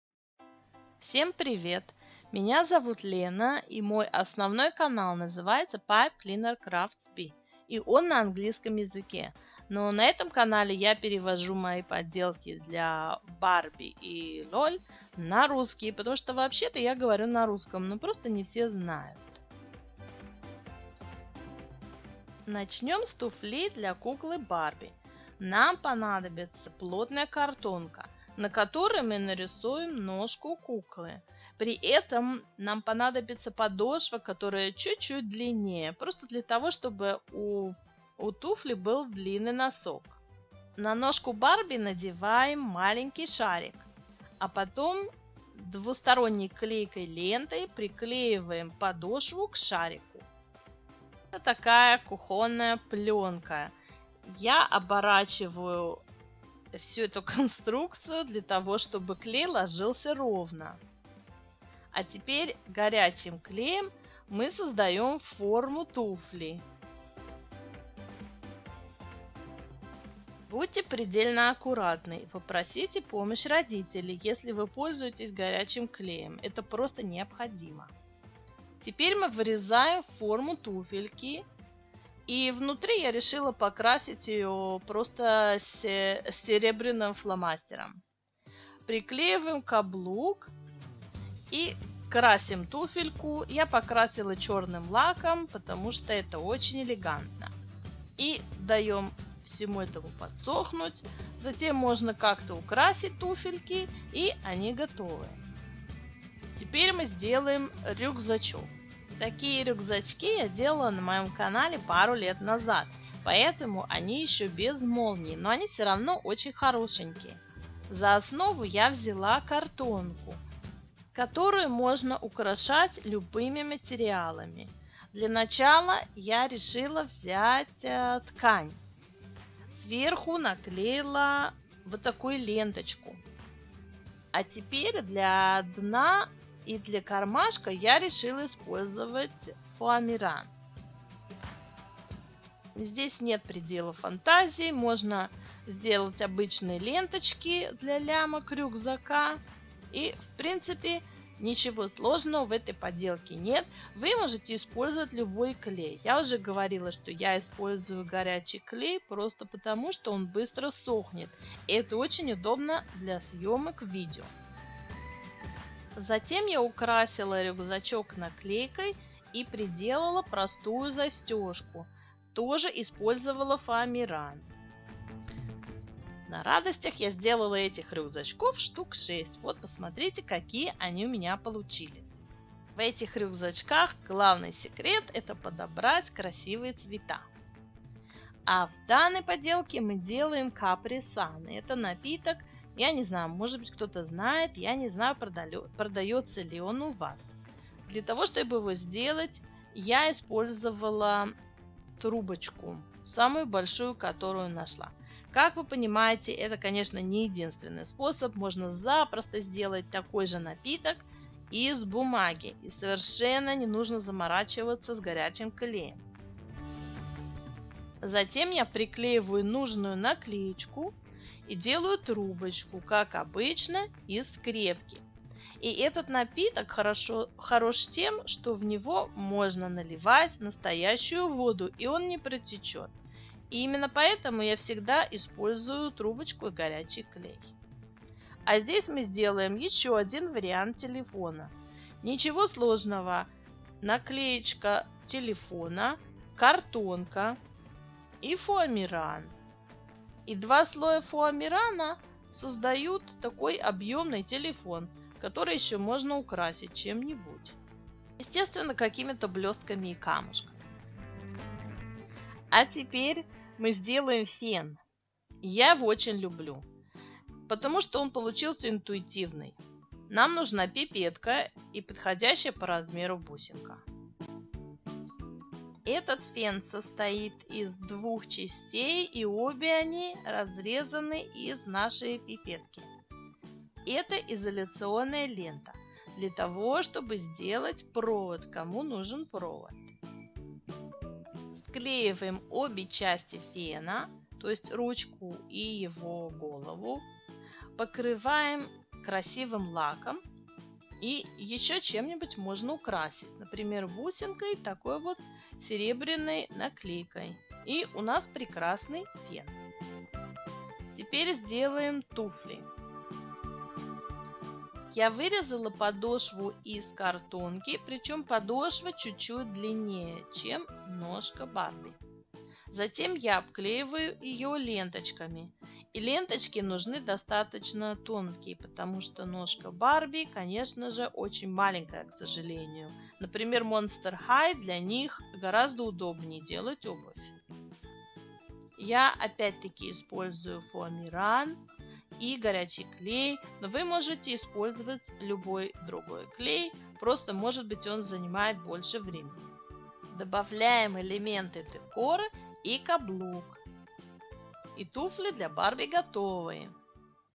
14 Лайфхаков для барби ~ мультик барби
14 Лайфхаков Мини телефон, туфли  напитки, кепка, рюкзачки и ещ. фен, швабра, как делать пенал и школьные пренадлежности
diy miniature school supplies / мультик барби
лайфхаки для барби